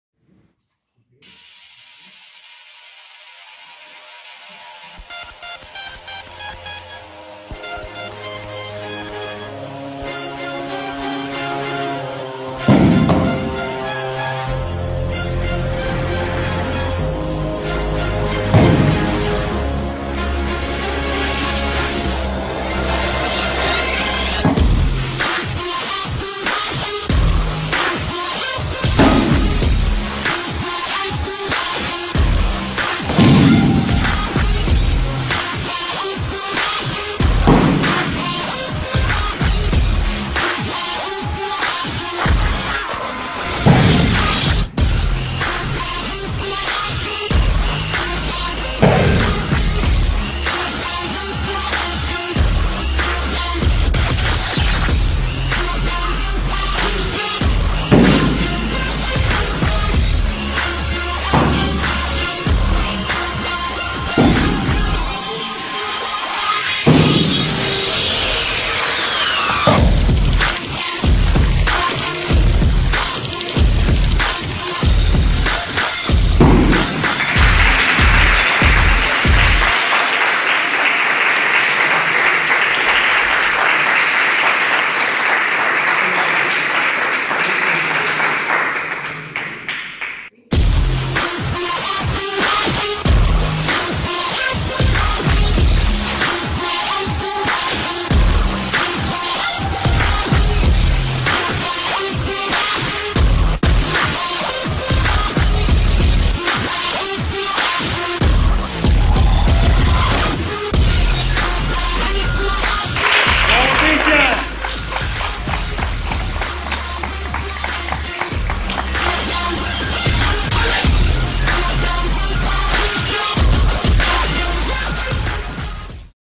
Cristian 6 anni una pura forza della natura durante l'esame di Ju Jitsu - G.A.DIURNO
Cristian una pura forza della natura, a soli 6 anni è l'incarnazione di Determinazione, Forza di Volontà e Tenacia. Sarà pura emozione vederlo sostenere l'esame di cintura verde di Ju Jitsu, il perché lo capirai alla fine di questo video...